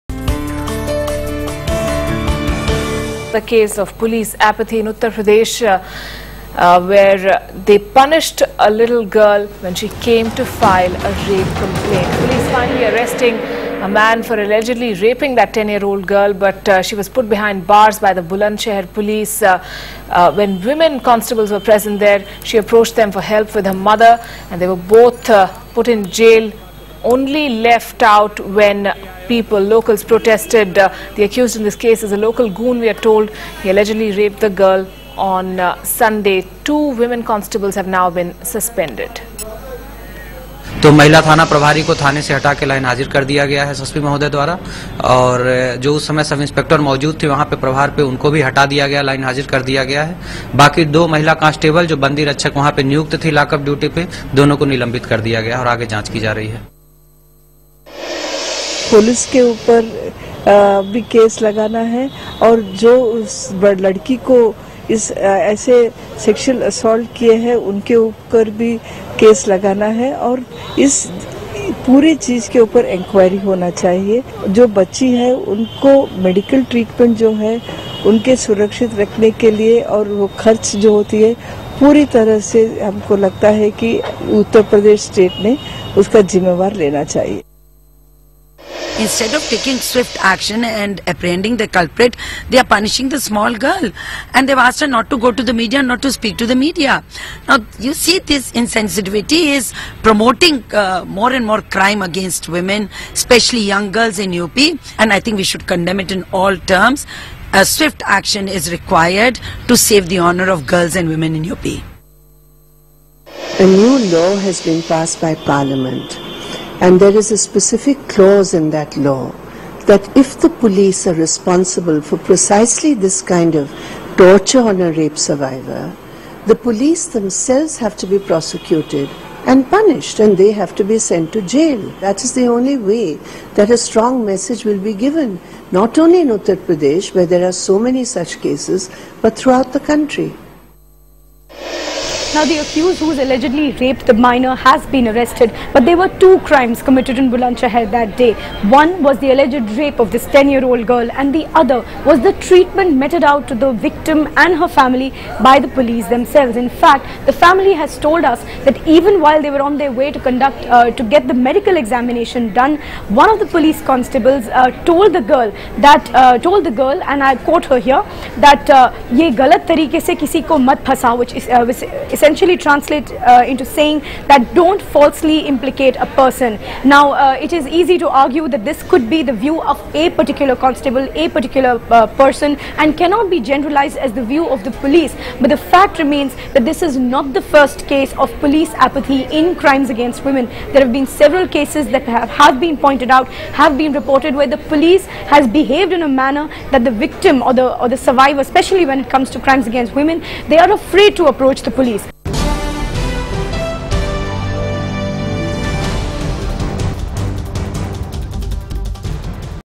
Police arrest man accused of raping minor
Police on Tuesday arrested a man for allegedly raping a 10-year-old, who was put behind bars when she and her mother went to report the crime. The child and her mother, who spent several hours behind the bars, were freed only after locals created an uproar.